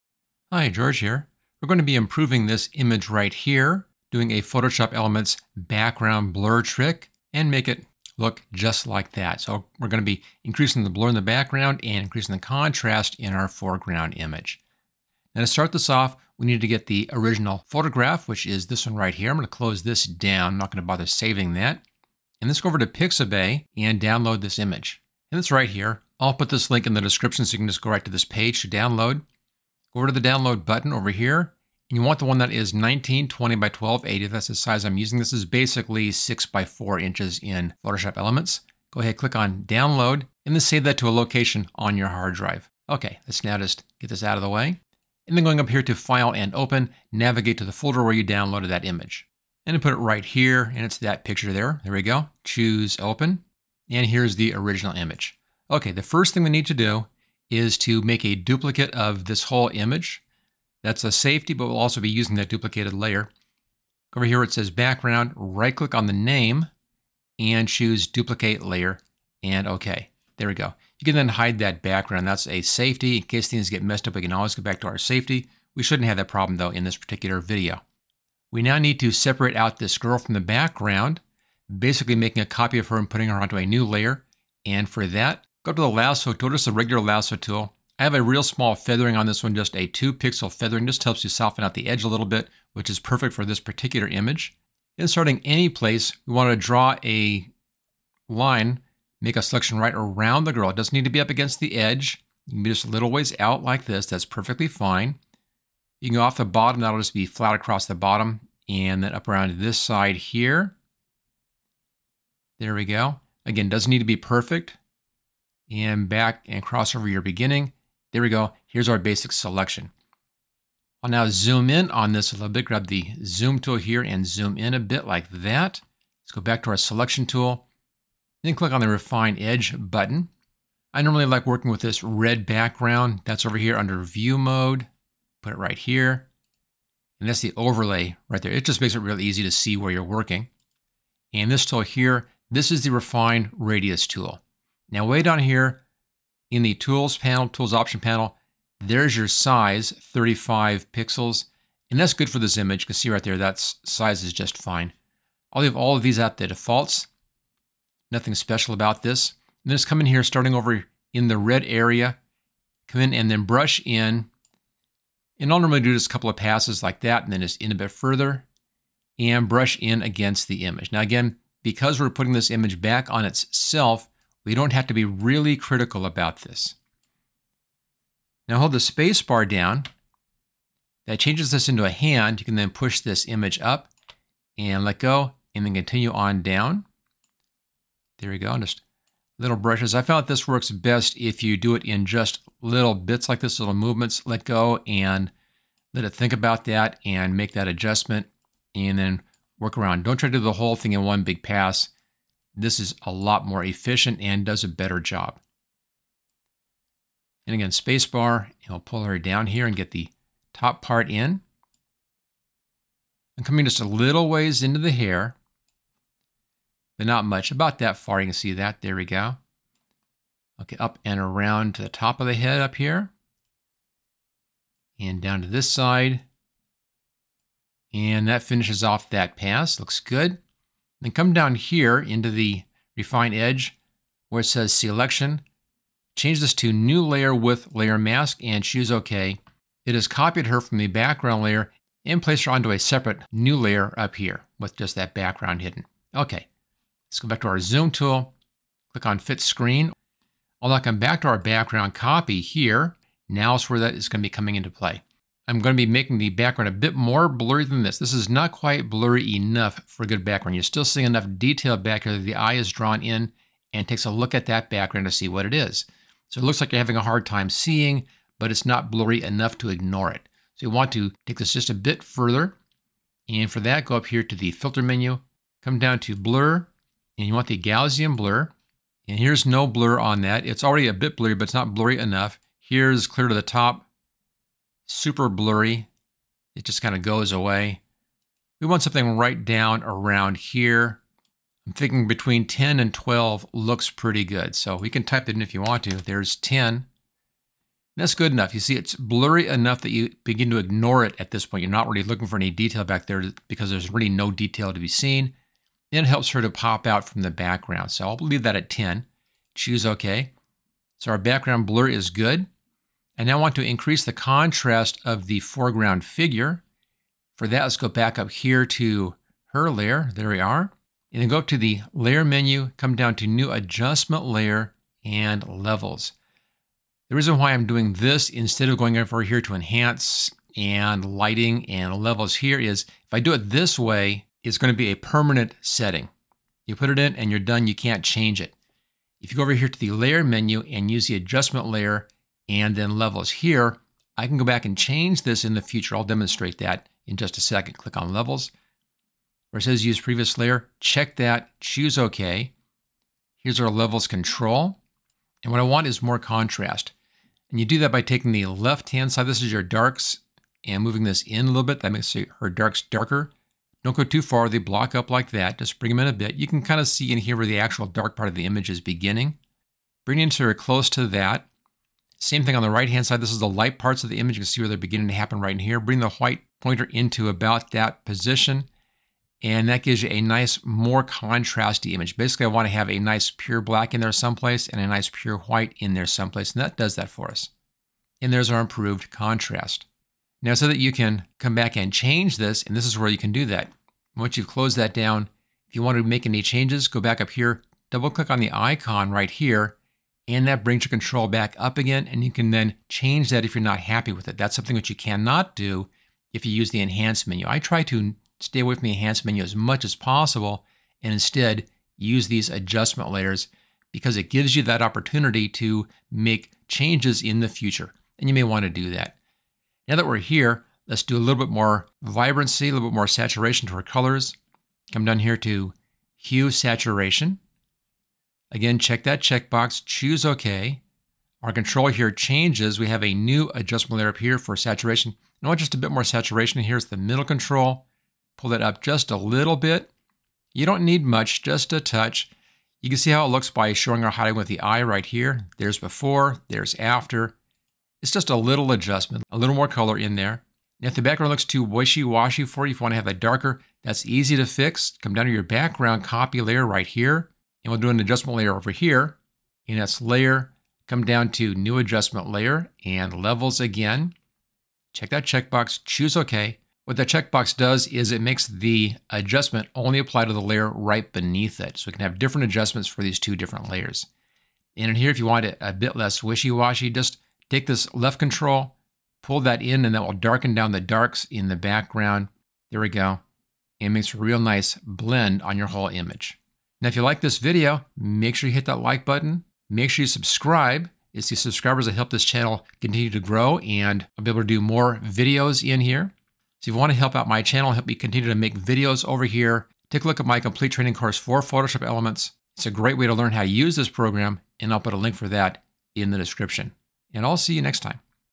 How You Can Use Photoshop Elements to Blur a Background for a Fast Shallow Depth of Field Effect
I show you how you can Create a Photoshop Elements blur background limited focus Glamour Photography portrait shot effect.

Blur background Photoshop Elements. Photoshop Elements Background Blur. In this Photoshop Elements demonstrate using a Gradient Layer Mask, multiple Layers, and the Correct Camera Distortion Filter to create a Glamour Photography Look on a Portrait.

Chapters:
00:00 Photoshop Elements Blur Background
00:21 Download photo
00:51 Duplicate layer
01:17  Copy subject to new layer
04:10 Blur background
05:12 Increase subject contrast
06:59 Increase subject color saturation
07:30 Adjust background contrast
08:06 Like Subscribe